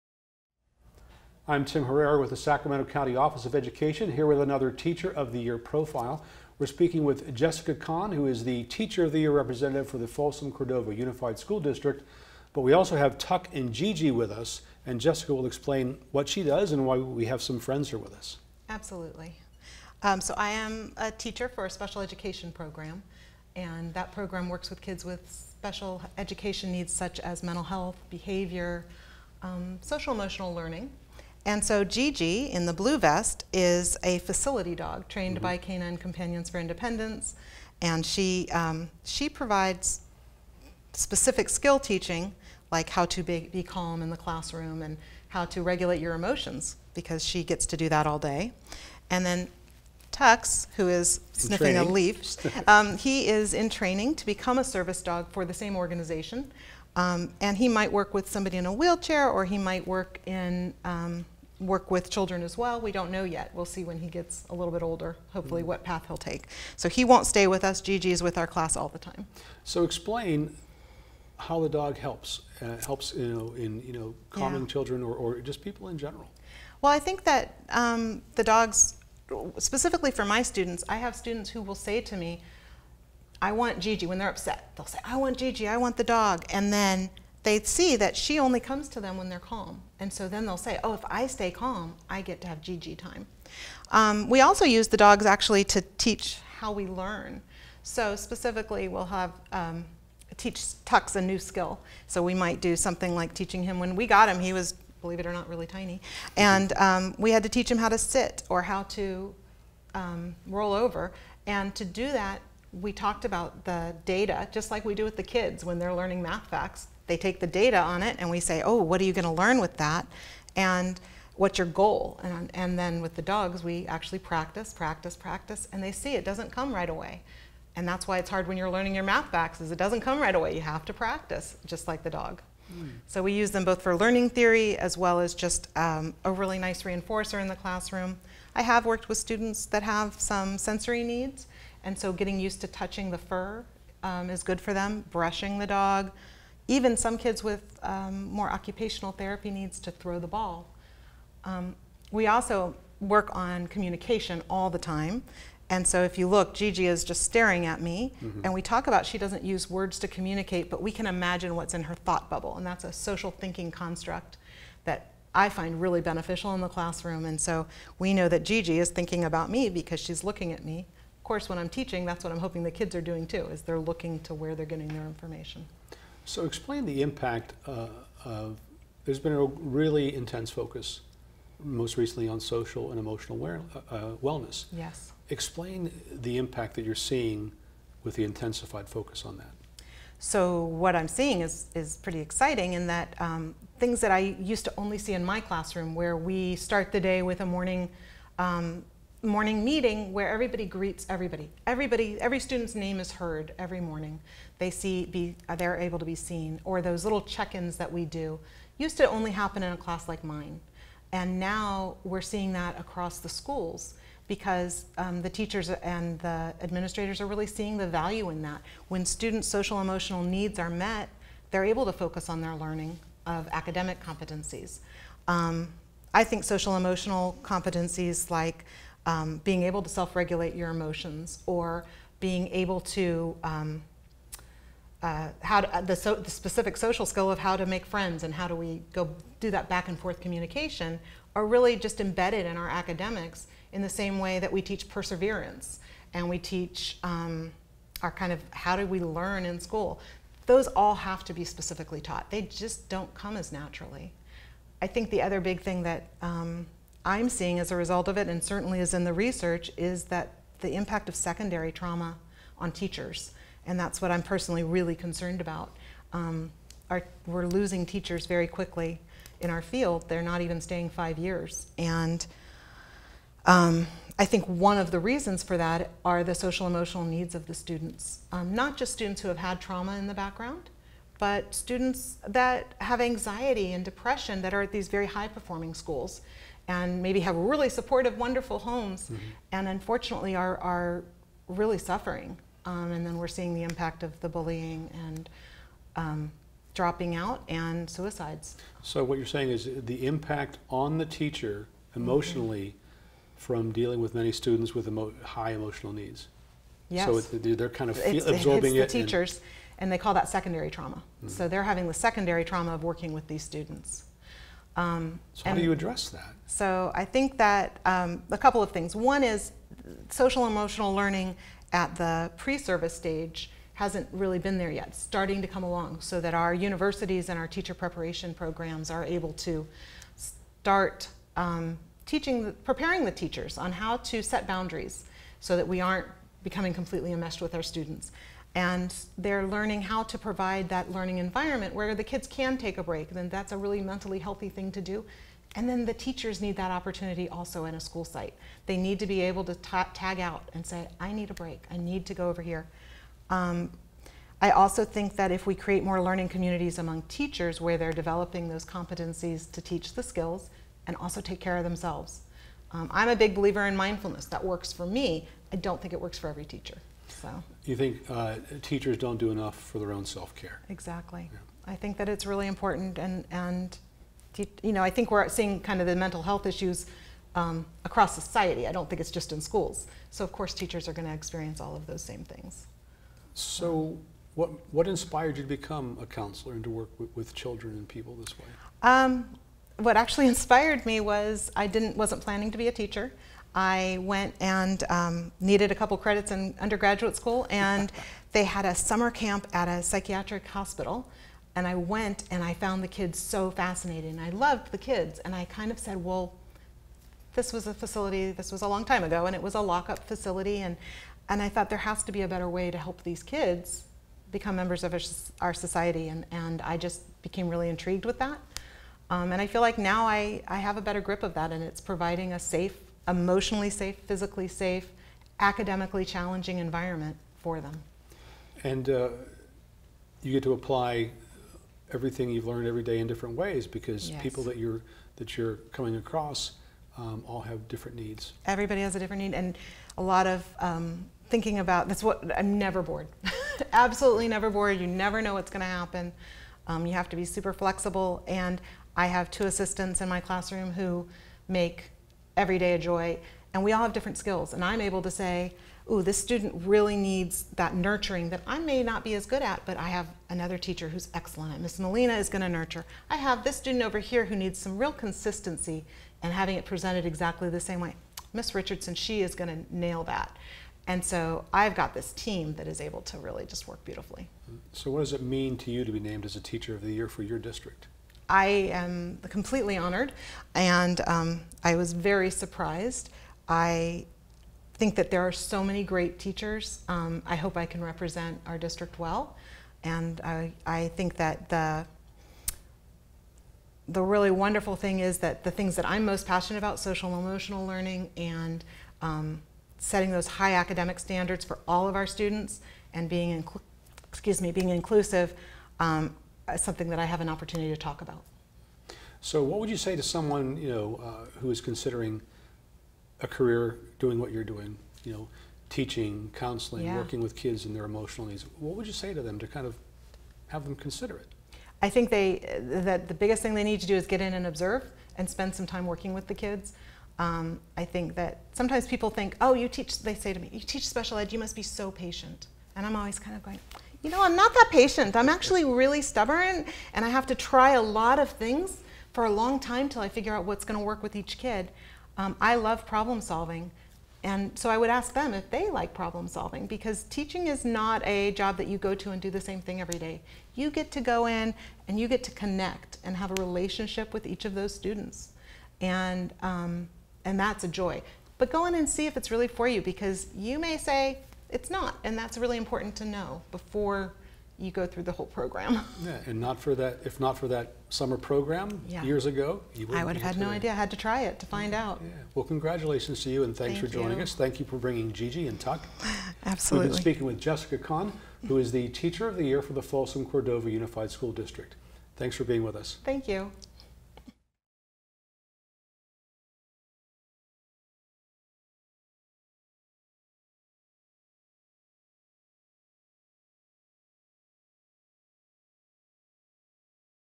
Jessica Kahn: Sacramento County Teachers of the Year 2020 Interview
An interview with Jessica Kahn, the Folsom Cordova Unified School District Teacher of the Year 2020. The Sacramento County Teachers of the Year program recognizes excellence in teaching within the county's schools. Participating districts nominate individual teachers to receive this special recognition. At the Teachers of the Year awards recognition dinner, two teachers are identified as the Sacramento County Teachers of the Year and are then eligible to participate in the statewide competition.

Comments made in this video don't necessarily represent SCOE's positions, strategies, or opinions.